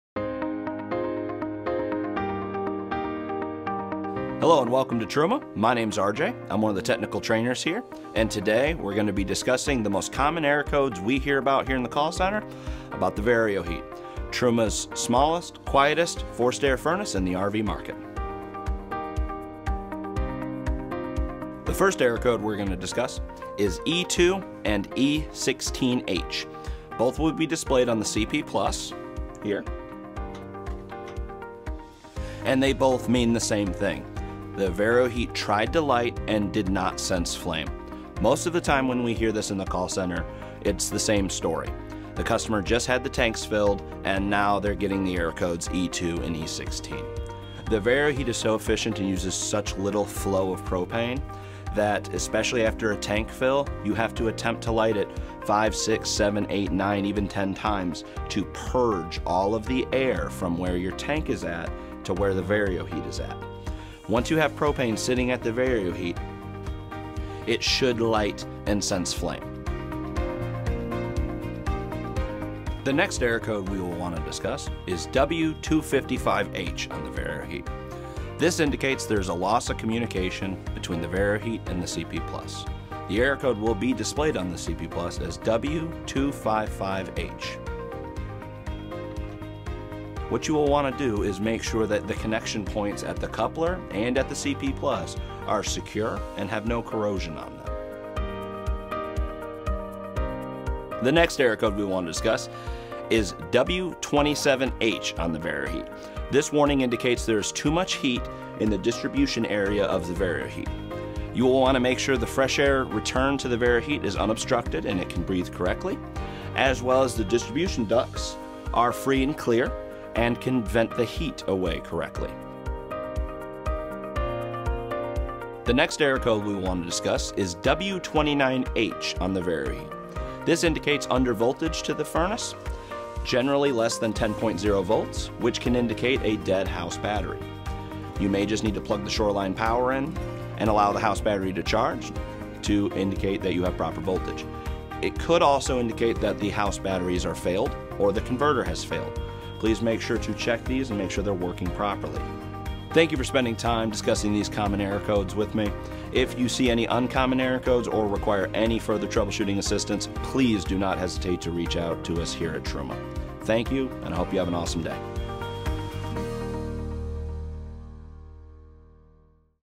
Truma VarioHeat Tech Tip: Understanding Common Error Codes
In this Truma Tech Tip, Technical Trainer RJ Mewton walks us through the meaning and troubleshooting of common VarioHeat Error Codes. For general information on the Truma VarioHeat, please